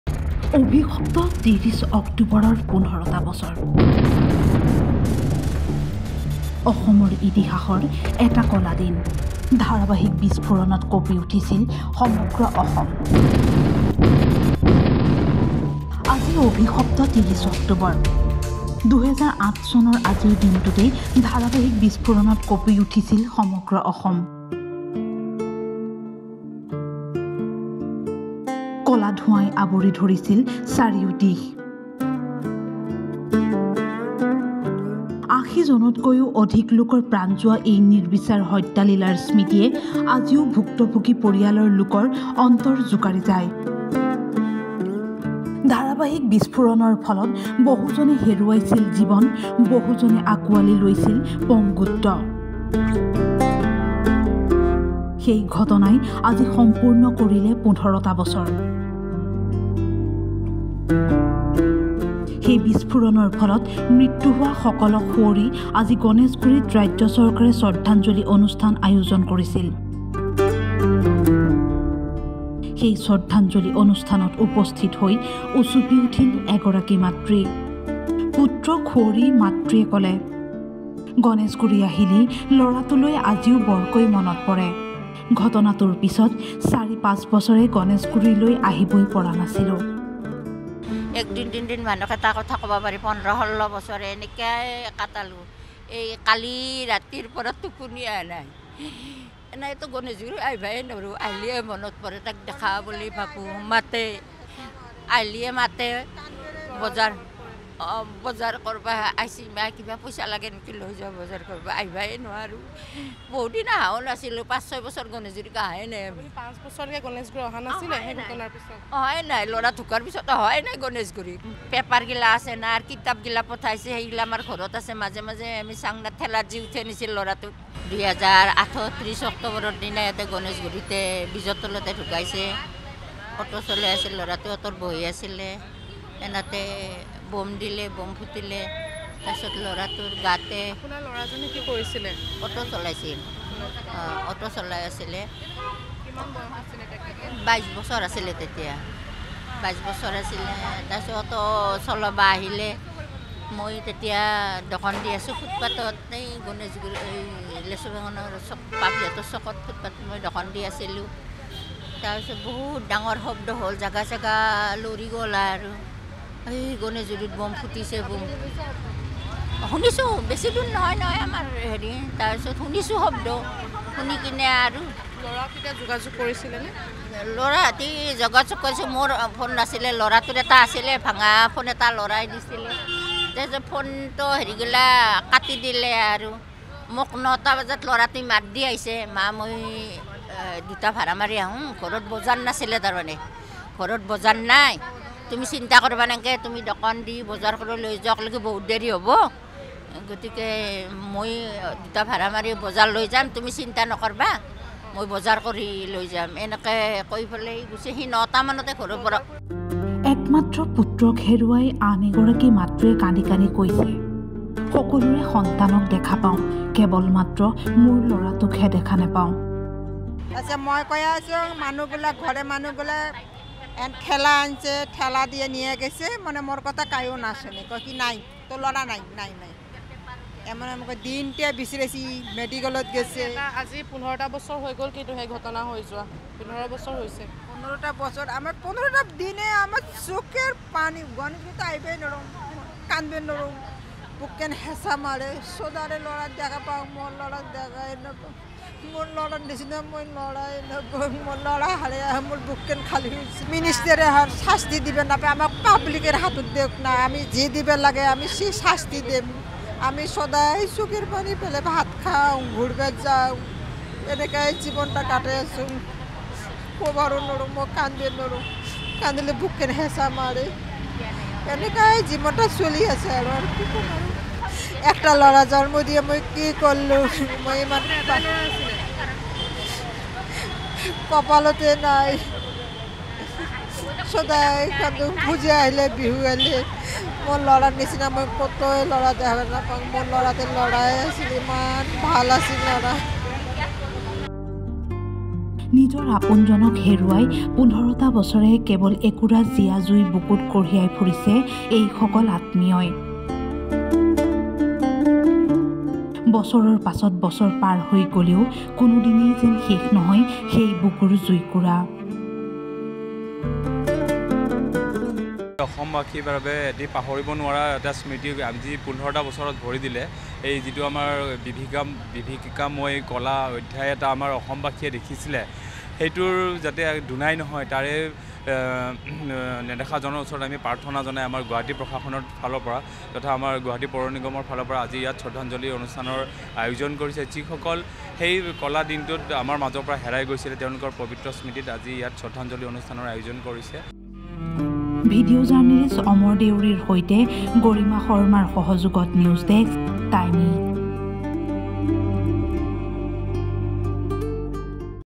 অভিশপ্ত ৩০ অক্টোবৰ
অভিশপ্ত ৩০ অক্টোবৰ। ভয়ংকৰ ঘটনাৰ বলি হোৱা লোকসকলক সুঁৱৰি গণেশগুৰি উৰণীয়া সেতুৰ তলত ৰাজহুৱা শ্ৰদ্ধাঞ্জলি অনুষ্ঠান। শ্ৰদ্ধাঞ্জলি অনুষ্ঠানত পুনৰ উচুপি উঠিল ভুক্তভোগীৰ পৰিয়াল।